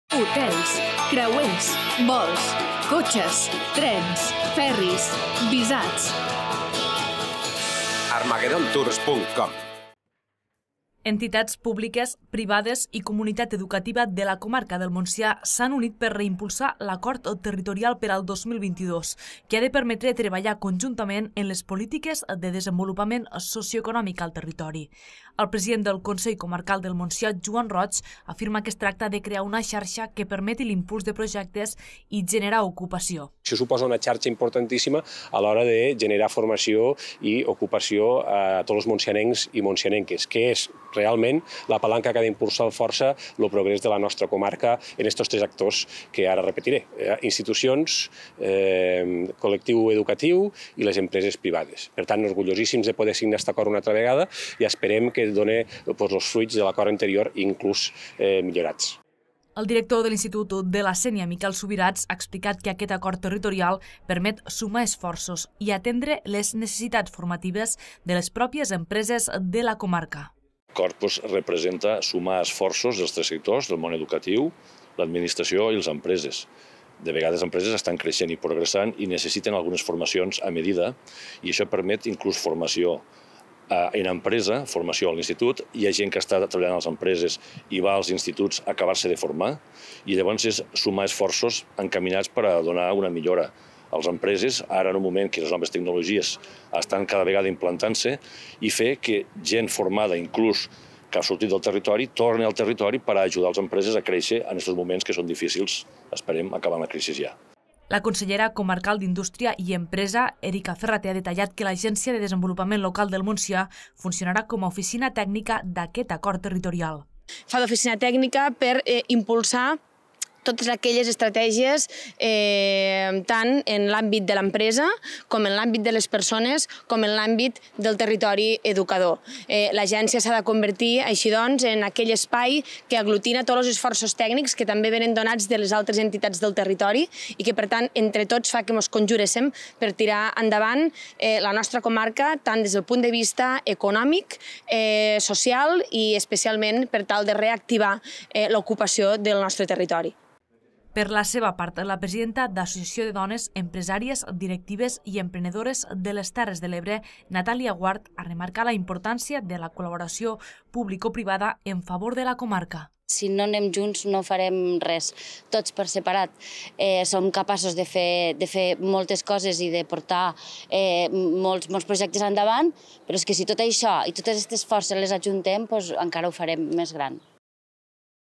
El Montsià reimpulsa l’Acord Territorial per al desenvolupament socioeconòmic de la comarca
Entitats públiques, privades i comunitat educativa de la comarca del Montsià s’han unit per reimpulsar l’Acord Territorial per al 2022, que ha de permetre treballar conjuntament en les polítiques de desenvolupament socioeconòmic al territori. El president del Consell Comarcal del Montsià, Joan Roig, afirma que es tracta de crear una xarxa  que permeti l’impuls de projectes i generar ocupació.

El director de l’institut de la Sénia, Miquel Subirats, ha explicat que aquest Acord Territorial permet sumar esforços i atendre les necessitats formatives de les pròpies empreses de la comarca.

La consellera comarcal d’Indústria i Empresa, Èrika Ferraté, ha detallat que l’Agència de Desenvolupament Local del Montsià funcionarà com a oficina tècnica d’aquest Acord Territorial.

Per la seva part, la presidenta d’Associació de Dones Empresàries, Directives i Emprenedores de les Terres de l’Ebre (ADEDE), Natàlia Guart, ha remarcat la importància de la col·laboració público-privada en favor de la comarca.